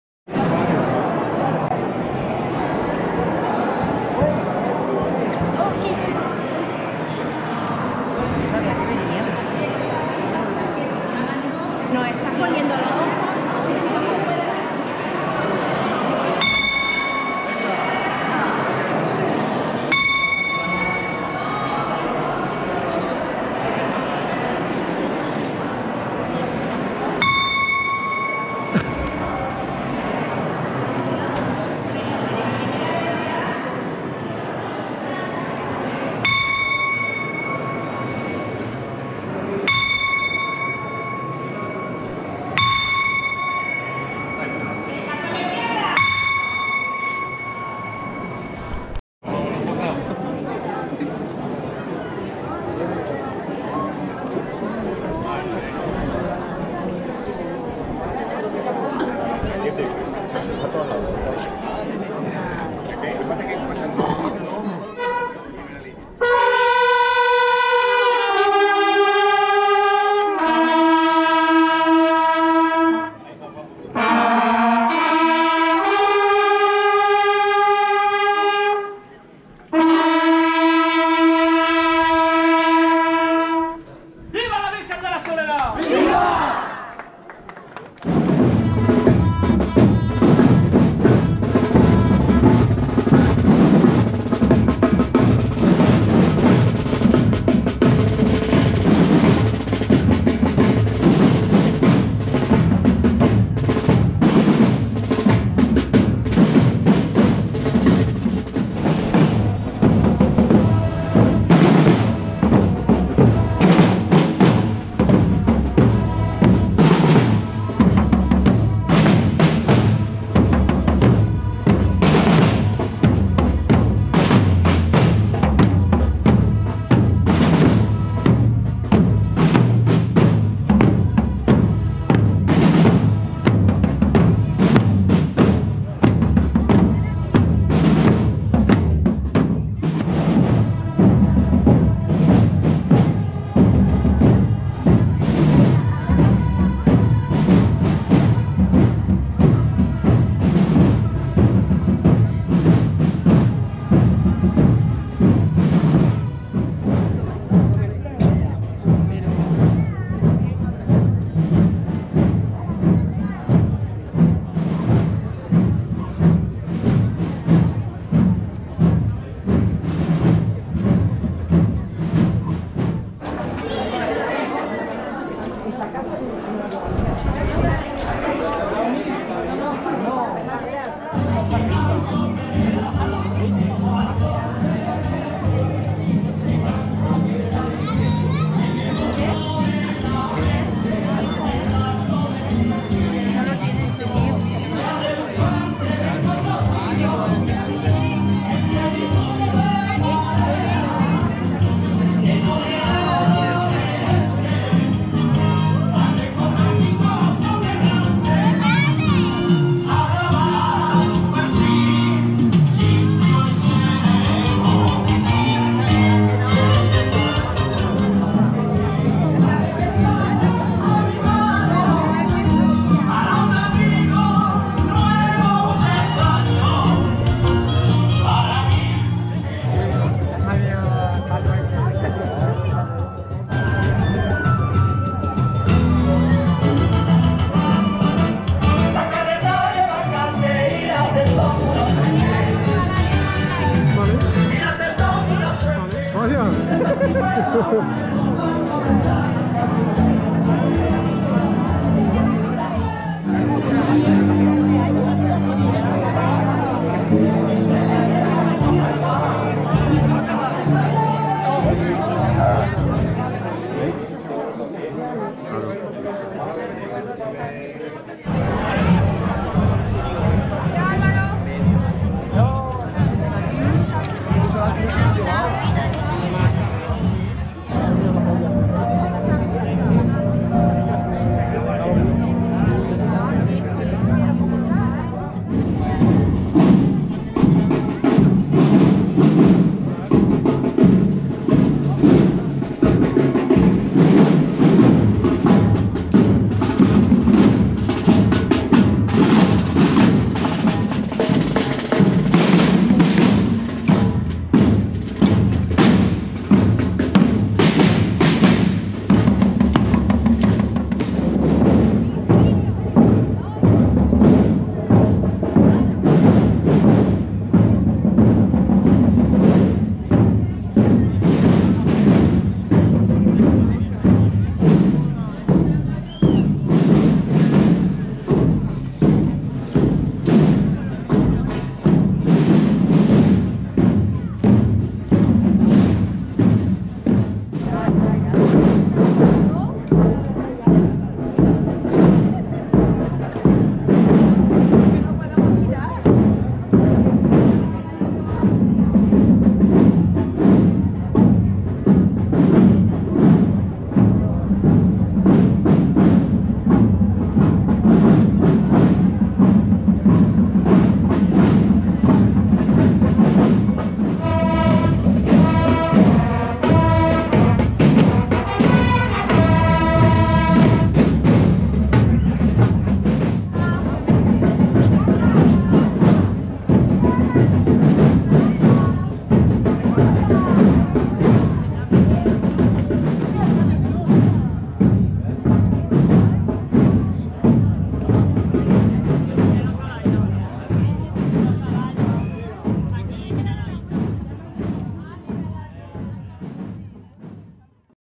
FIESTAS ASCENSIÓN 2022. 4k
Virgen de la Soledad  procesionando por su  barrio de San Millán con motivo de la festividad de la Ascensión 2022